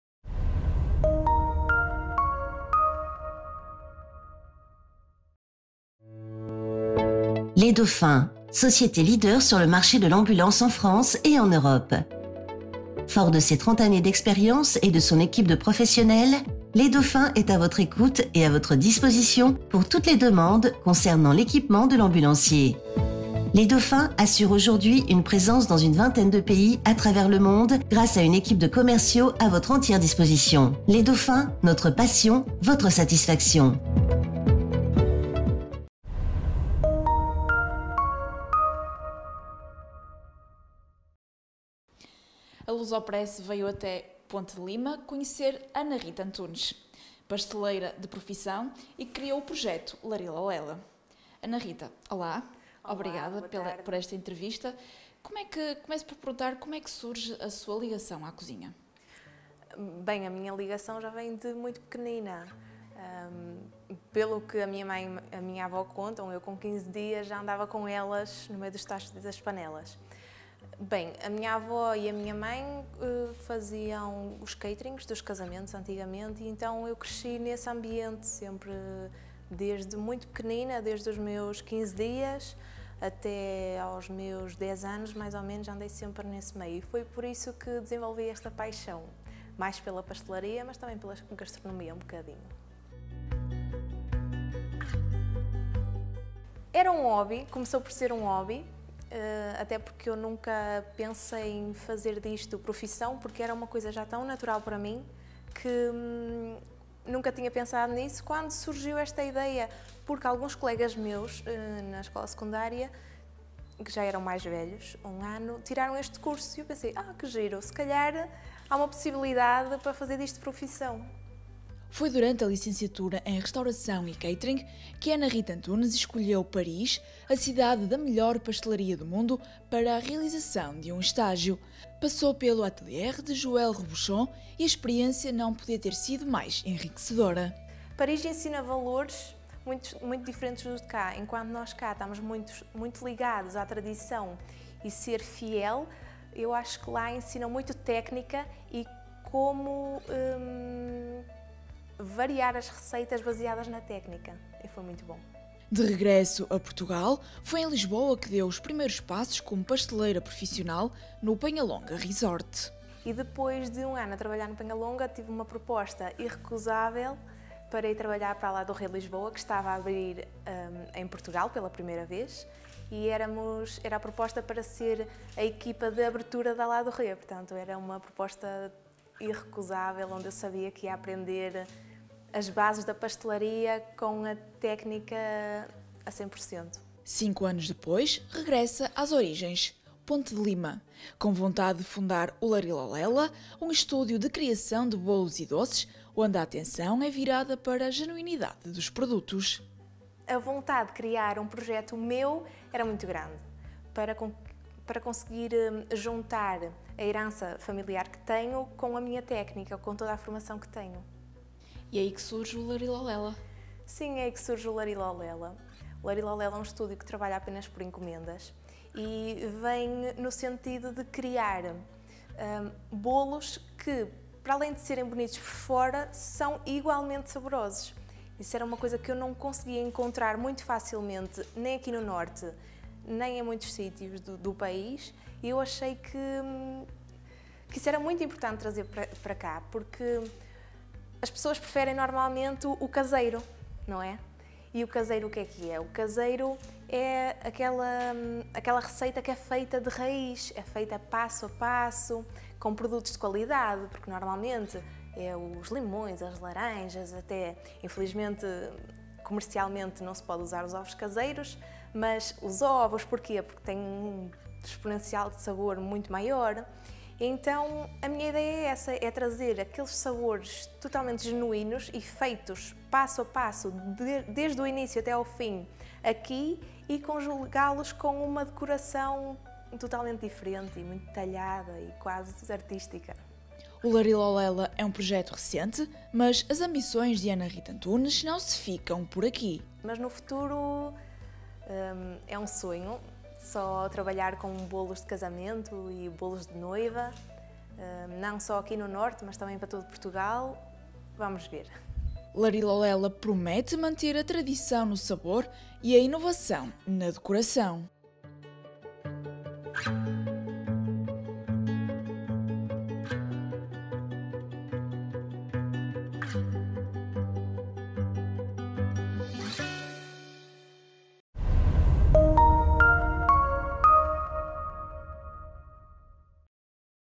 Especial região do Minho: pasteleira profissional Ana Rita Antunes criou o projeto Larilolela
Nasceu entre panelas e tachos, e isso influenciou o seu trajeto. Foi durante a licenciatura em Restauração e Catering que Ana Rita Antunes escolheu Paris, a cidade da melhor pastelaria do mundo, para a realização de um estágio. Passou pelo Le Atelier de Joel Robuchon e a experiência não podia ter sido mais enriquecedora. De regresso a Portugal, foi em Lisboa que deu os primeiros passos como pasteleira profissional no Penha Longa Resort. Cinco anos depois, regressa às origens: Ponte de Lima, com vontade de fundar o Larilolela, um estúdio de criação de bolos e doces, onde a atenção é virada para genuinidade dos produtos. O Larilolela é um projeto recente, mas as ambições de Ana Rita Antunes não se ficam por aqui. Larilolela promete manter a tradição no sabor e a inovação da decoração.